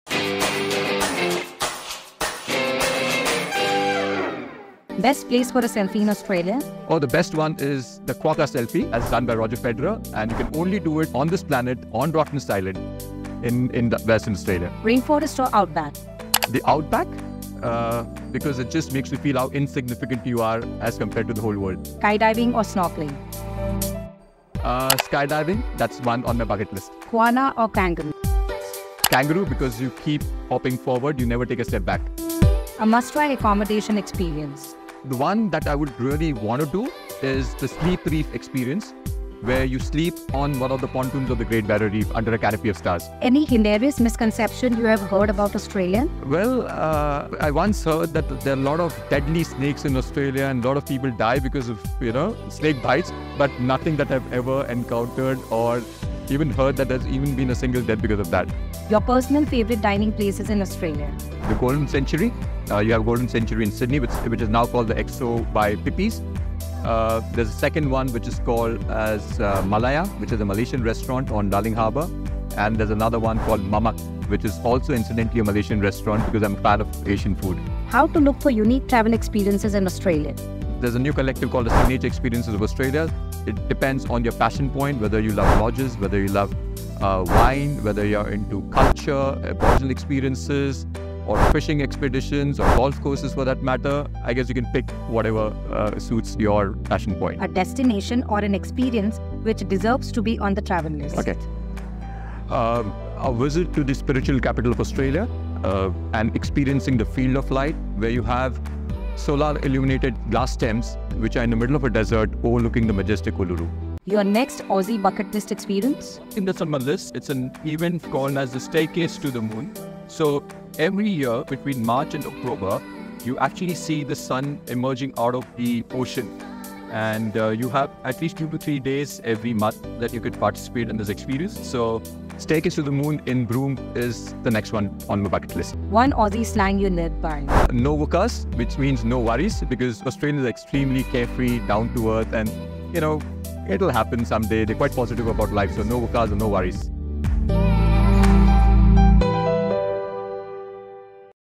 Mastery in Minutes with Nishant Kashikar Tourism Australia
Mastery in Minutes: In an exclusive interview, we delve into the insights of Nishant Kashikar, the Country Head for India & Gulf at Tourism Australia. Beyond the conventional travel guides and training programs, Nishant shares his perspective on Australia, offering a unique glimpse into what makes the land down under so captivating for him.